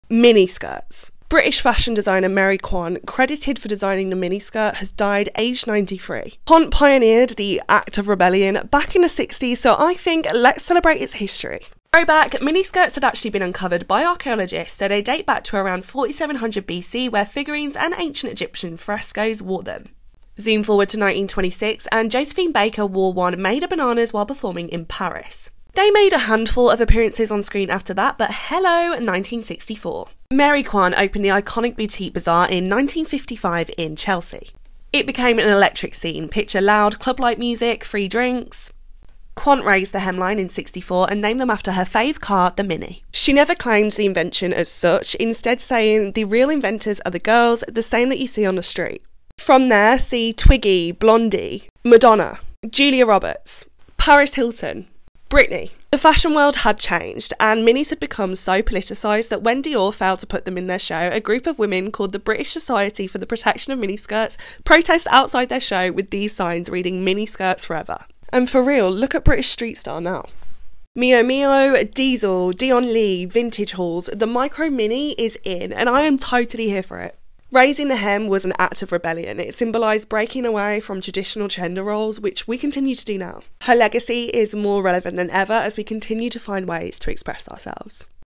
The History Of The Mini Skirt & Where It Is Now | Mary Quant
Mini skirts.
British fashion designer Mary Quant, credited for designing the miniskirt, has died aged 93.
Quant pioneered the ‘act of rebellion’ back in the 60s so I think let’s celebrate its history.
Throwback. Miniskirts had actually been uncovered by archaeologists so they date back to around 4700 B.C. where figurines and Ancient Egyptian frescos wore them.
Zoom forward to 1926 and Josephine Baker wore a one made of bananas while performing in Paris.
They made a handful of appearances on screen after that but hello, 1964.
Mary Quant opened the iconic boutique Bazaar in 1955 in Chelsea.
It became an electric scene, picture loud, club-like music, free drinks.
Quant raised the hemline in 64’ and named them after her fave car, mini.
She’s never claimed the invention as such, instead saying: “The real creators of miniskirt are the girls, the same that you seen in the streets”
From there, see: Twiggy, Blondie, Madonna, Julia Roberts, Paris Hilton, Britney…
The fashion world had changed. At once point, minis became so politicised that when Dior failed to show them in their collection, a group of women known as the ‘British Society for the Protection of Mini Skirts’ protested outside the show with signs reading ‘mini skirts forever’.
And for real, look at street style now.
Miu Miu, Diesel, Dion Lee, Vintage hauls. The mini is now a micro mini and I cannot get enough of it.
Raising the hem was an act of rebellion; it symbolised breaking away from traditional gender roles which we continue to do now.
Her legacy is more relevant than ever as we continue to find ways to express ourselves.